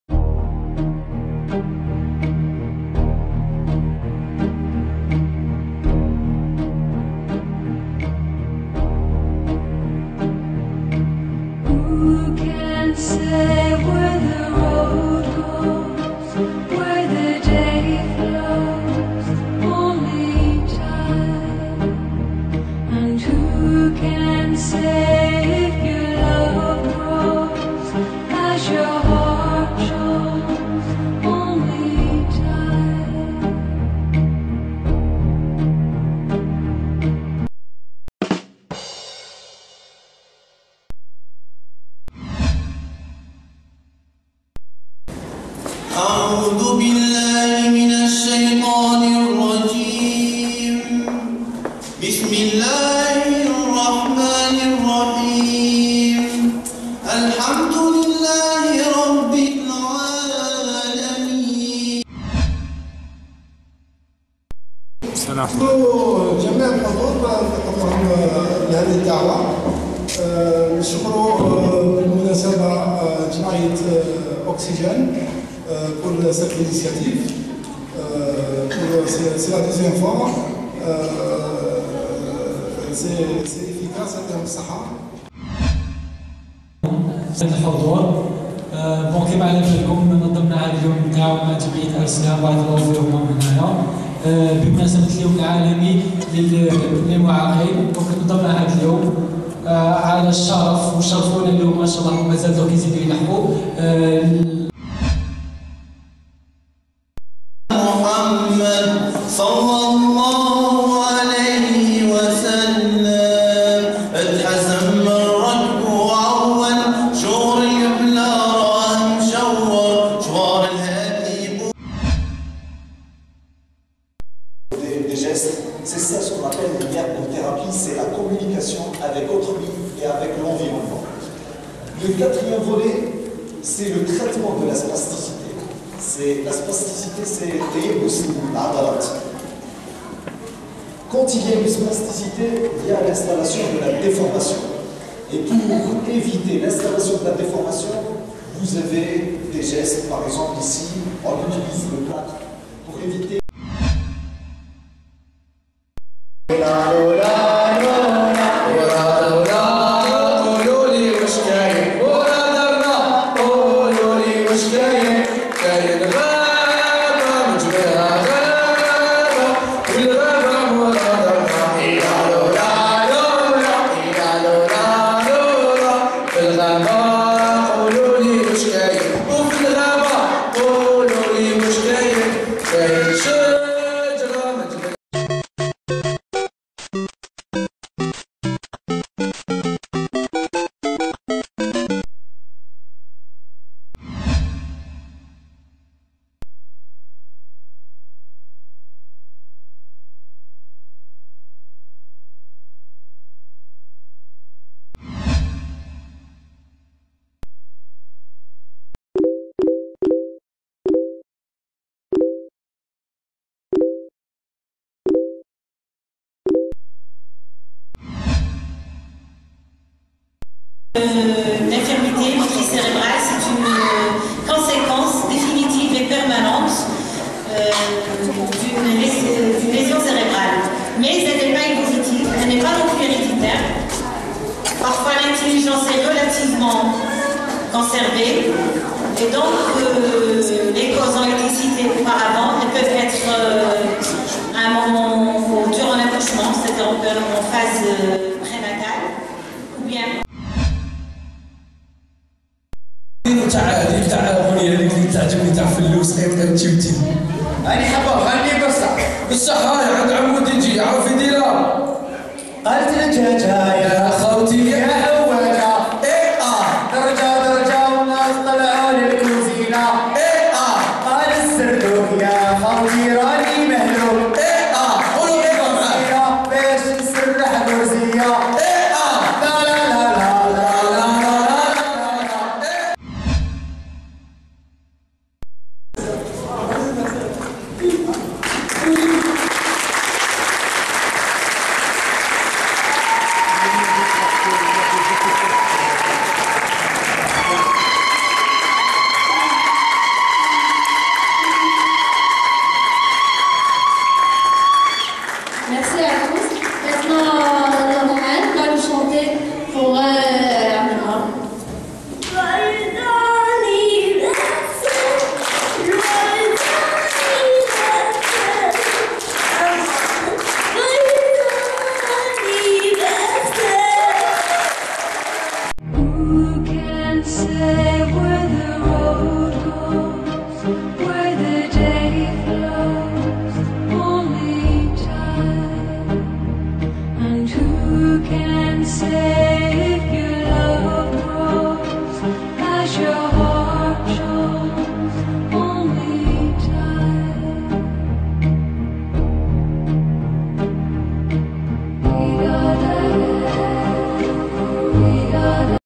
Handicapé mais je survie
Journée, pour les enfants souffrant d'IMC, organisée par le groupe Oxyjeunes Blida et l'association Arslen le 3 décembre 2013 à la maison de Jeunes Bounaama Djilali - Sidi Abd El Kader - Blida

Pour rejoindre le groupe Oxyjeunes, veuillez consulter la page FB:
OxyJeunes Algérie - Blida -
Oubien contactez:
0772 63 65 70 / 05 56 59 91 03

Bienvenu